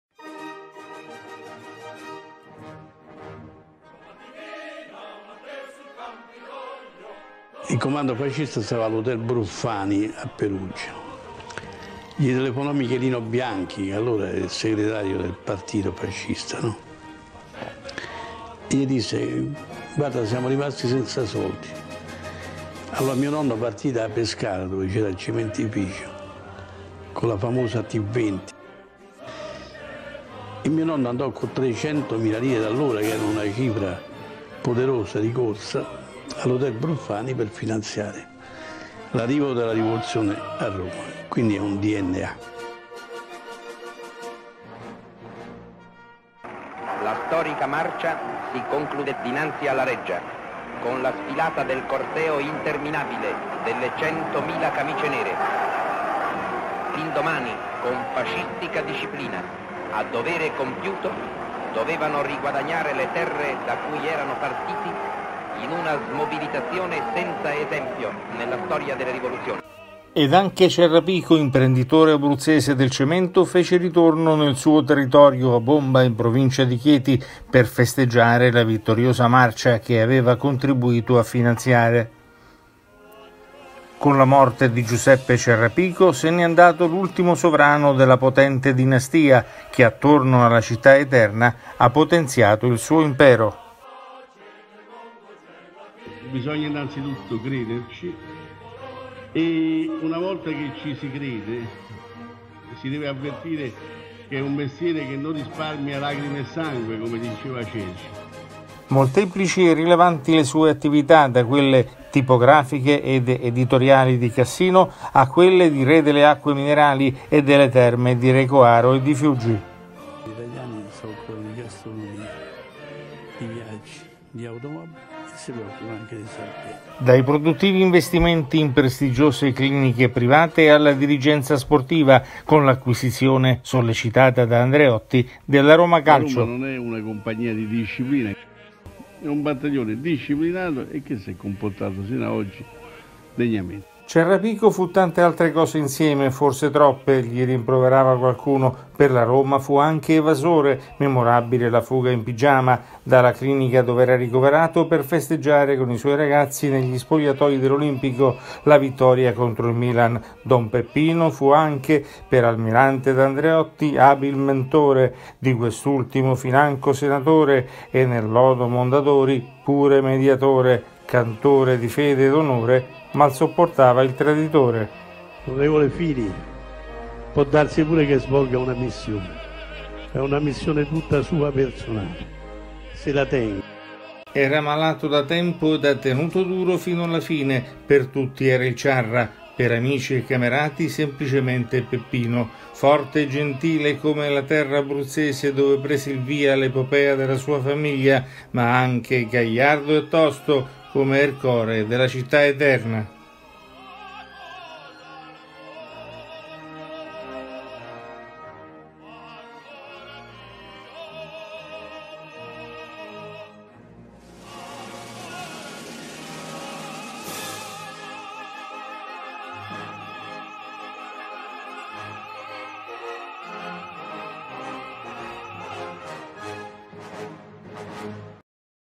CIARRAPICO: "LACRIME E SANGUE, POI VINCI". ADDIO AUDACE PIONIERE IN CAMICIA NERA!
GIUSEPPE CIARRAPICO, L' ORGOGLIO D'ESSERE FASCISTA.
LA SUA FAMIGLIA, IMPRENDITORI DEL CEMENTO DI ORIGINE ABRUZZESE, ERA VICINA AL POTENTE QUADRUMVIRO DEL DUCE, MICHELE BIANCHI.
DOPO LA FINE DEL REGIME SPETTERA' A PEPPINO CIARRAPICO  RILANCIARE L'ATTIVITA' IMPRENDITORIALE, ESTENDENDOLA CON SUCCESSO AD ALTRI CAMPI.
DALL' EDITORIA ALLE ACQUE TERMALI, DALLE CLINICHE PRIVATE ALLO SPORT. FINO ALLA POLITICA.
CON LA MORTE DEL "CIARRA" SCOMPARE L' ULTIMO RE DELLA DINASTIA IN CAMICIA NERA, CHE NON HA MAI RINNEGATO LA VERACE FEDE FASCISTA.

Il Blog di Domenico Di Carmine